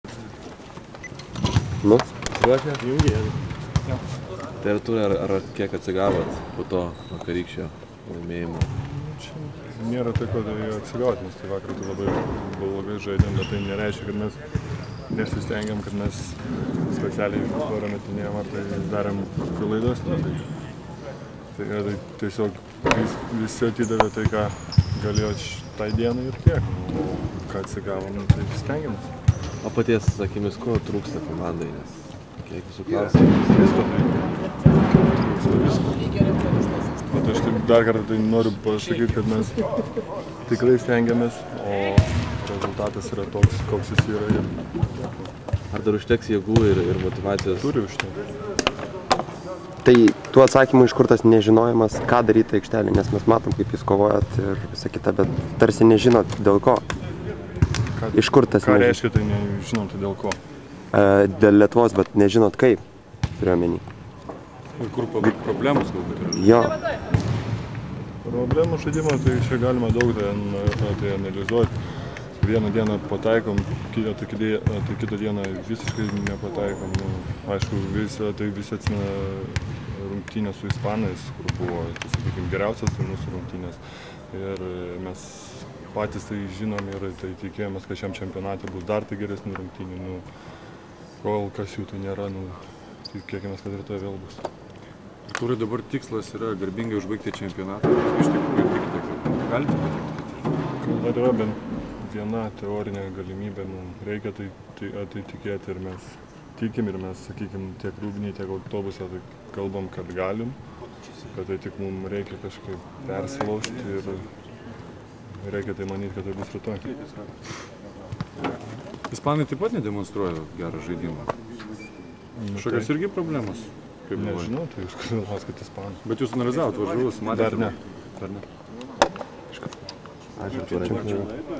Eurobasket.lt A.Jomantas: "Tikimes, kad galime rungtyniauti kaip su ispanais Vilniuje"
Lietuvos rinktinės krepšininkas Artūras Jomantas sekmadienį po rytinės treniruotės nebuvo labai kalbus. Rinktinės gynėjas tikino, kad reikia tikėti, jog galime petekti į kitą etapą ir susikoncentruoti rungtynėms su Ispanija.